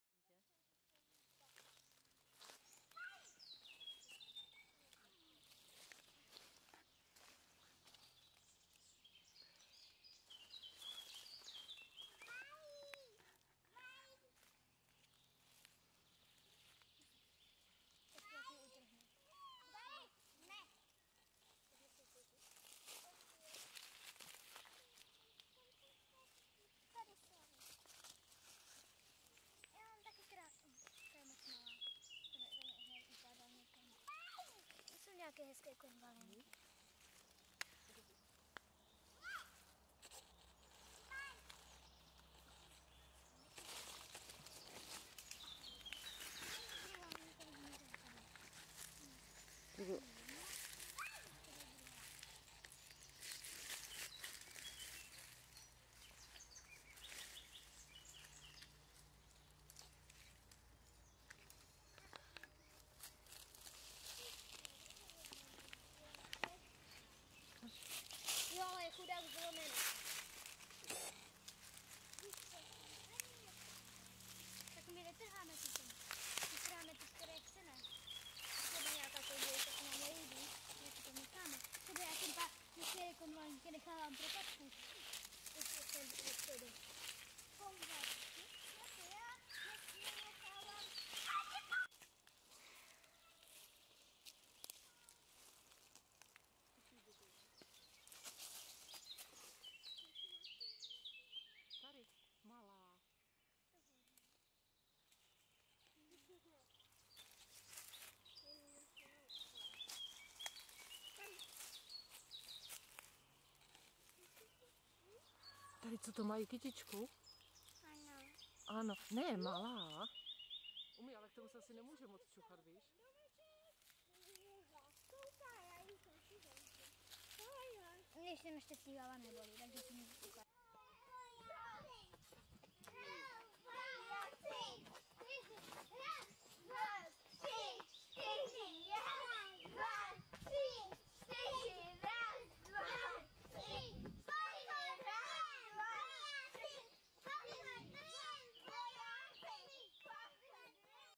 Medvědí česnek střídají konvalinky.
Medvědí česnek střídají konvalinky, jejich omamná vůně vylákala do lesa ve Strážovicích na Hodonínsku místní maminky s dětmi, jako za dob jejich maminek, babiček i prababiček, kdy to bývala takřka rodinná jarní tradice. Konvalinka vonná není chráněna a trhat ji můžeme prakticky všude, kromě chráněných území. Jelikož je tato kráska jedovatá, opatrnost byla samozřejmostí.